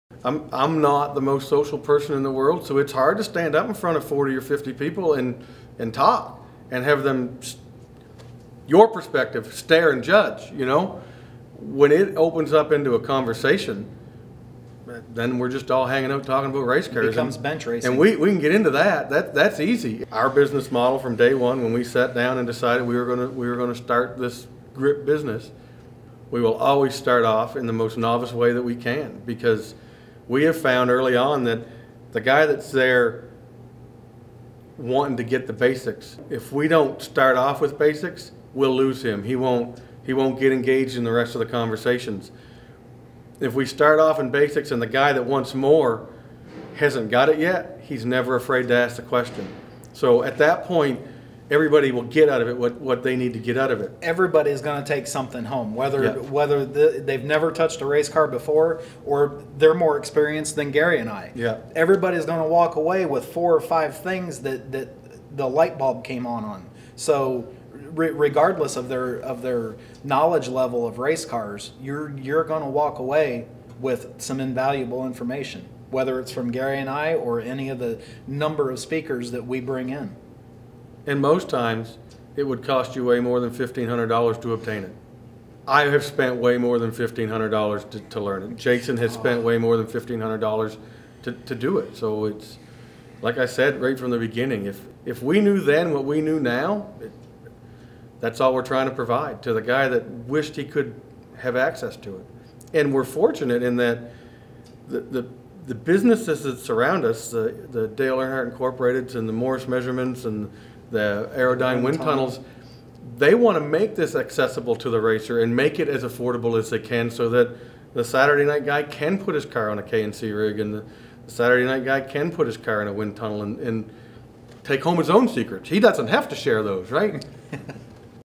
About GRIP Seminars with Jason Enders and Gary Crooks
Jason Enders and Gary Crooks explain the benefits of GRIP Seminars presented by Circle Track